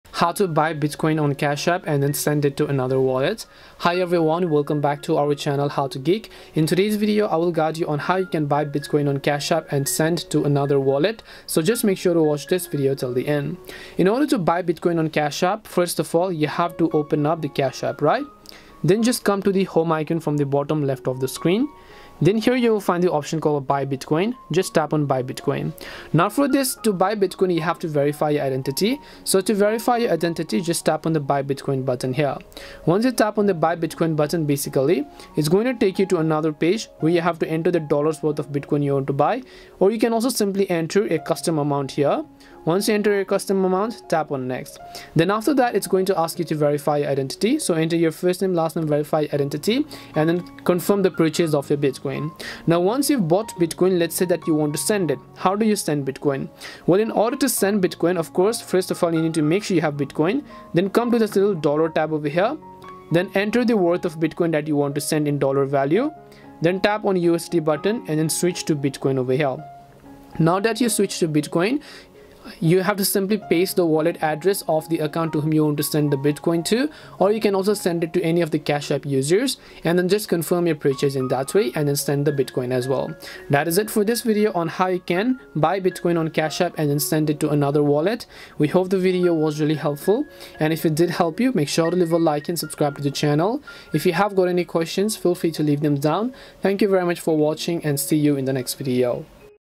How to Buy Bitcoin on Cash App and Send to Another Wallet
In this tutorial video, I will quickly guide you on how you can buy Bitcoin on Cash App and send it to another wallet. So make sure to watch this video till the end. We try to be responsive to as many questions as possible.

►  Chapters:
0:00 Introduction
0:13 Buy Bitcoin
0:56 Send Bitcoin
1:23 Conclusion